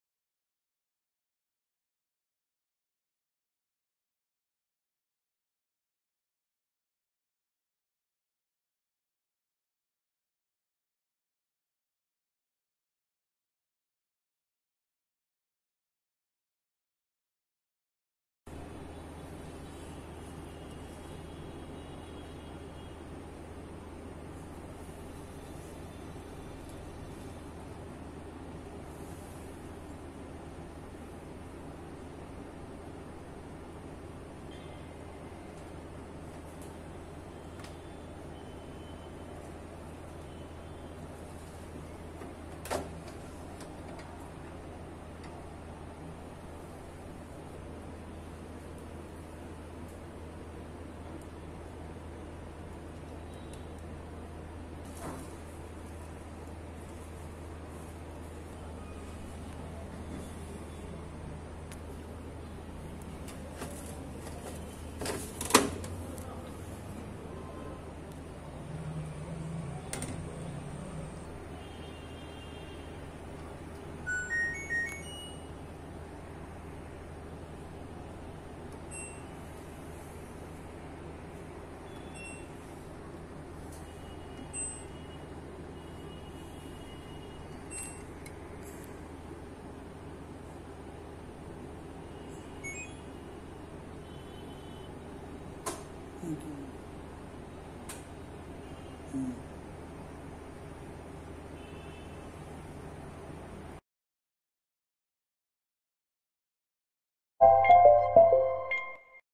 LG Top Load Washing Machine: Tub Clean Function Process
Cleaning the washer tub of your LG Top Load Washing Machine on a monthly basis using the Tub Clean function can help minimize the possibility of odors, detergent residue buildup and maximize your washing machine's performance. In this video, we will show you how to properly use the the Tub Clean feature on your LG washing machine.